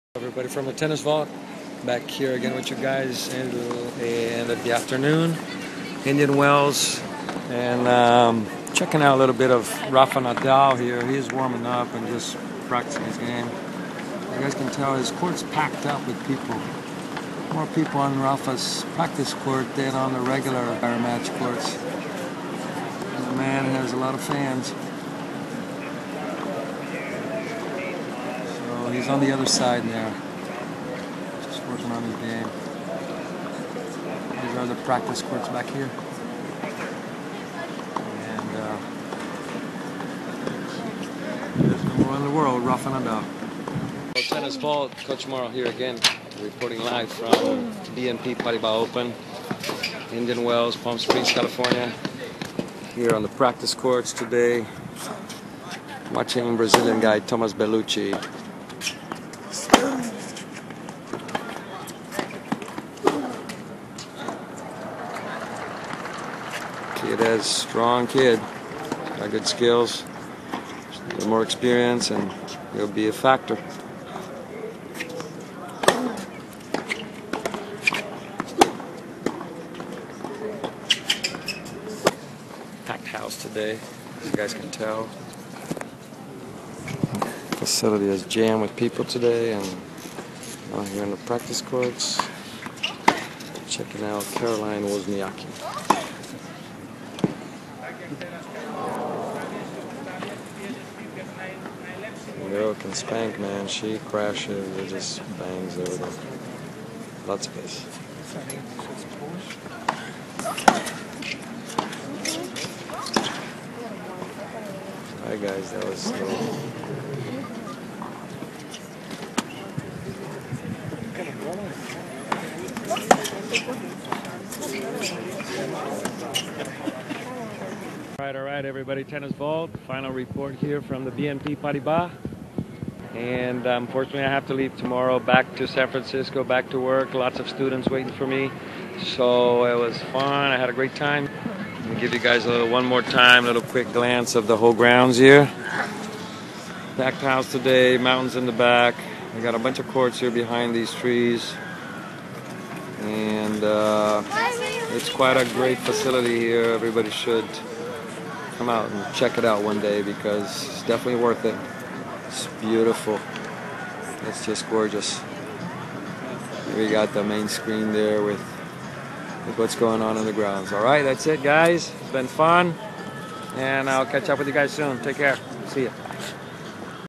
How To Play Tennis - Tennis Coach Mauro Marcos At The BNP Paribas Open 2011 in Indian Wells
Coach Mauro checks in one last time from the 2011 BNP Paribas Open in Indian Wells, CA, with some video of Rafael Nadal, Caroline Wozniacki and young Brazilian star Thomaz Bellucci on the practice courts.